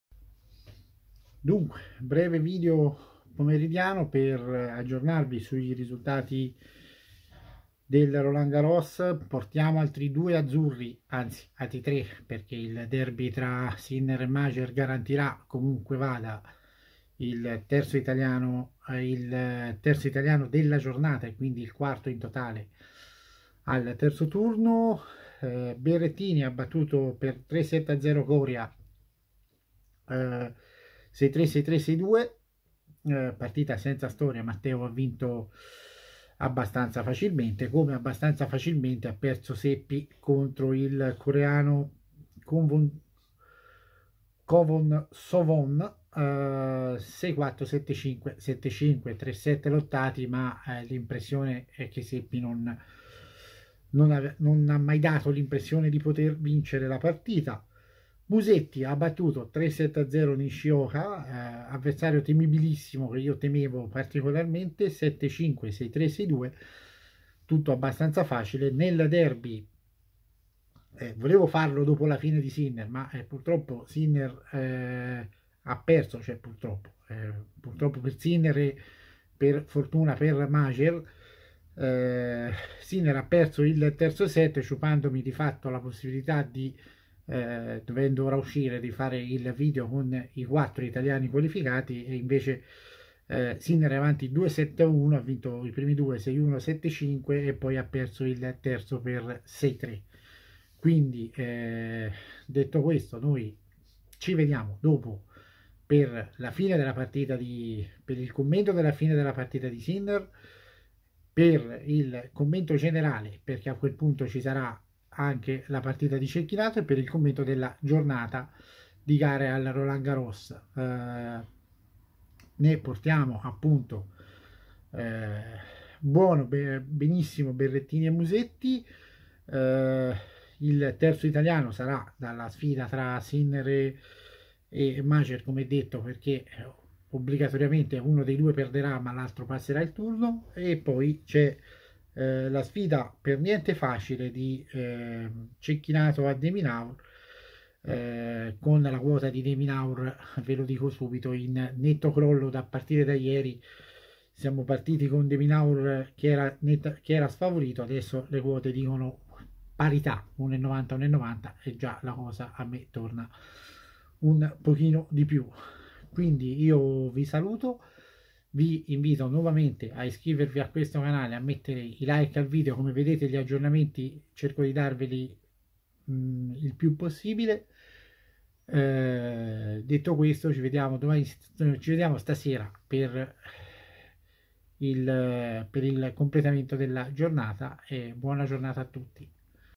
Berrettini-Coria 6-3 6-3 6-2 Musetti-Nishioka 7-5 6-3 6-2 . Fuori Seppi.
Berrettini e Musetti al terzo turno del rolandgarros. Saranno almeno 4 gli azzurri al terzo turno visto il derby in corso tra Sinner e Mager